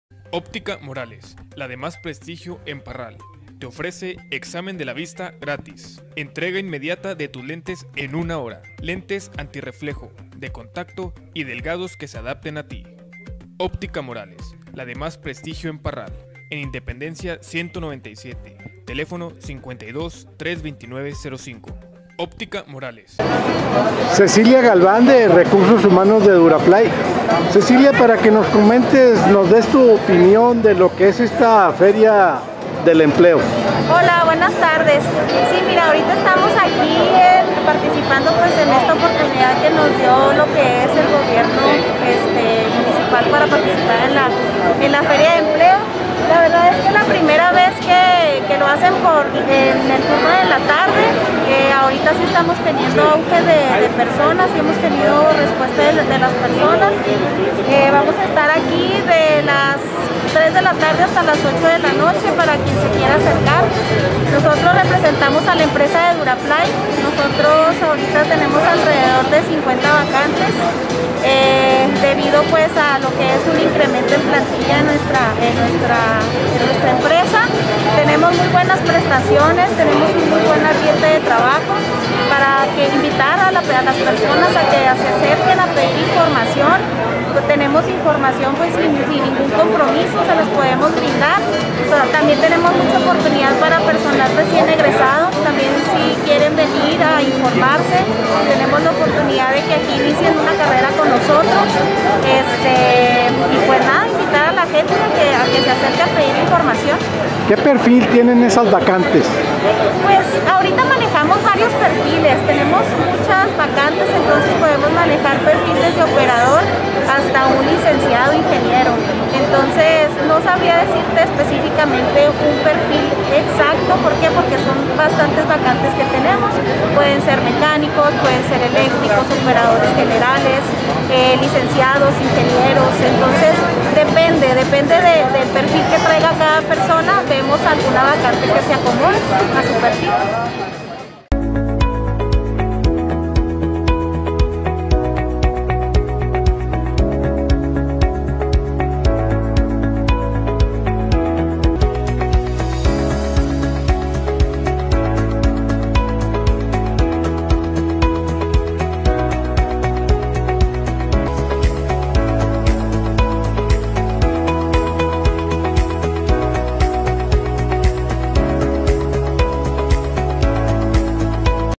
Cecilia Galván
Cecilia Galván de Recursos Humanos de Duraplay nos da a conocer las vacantes que existen para su planta de trabajo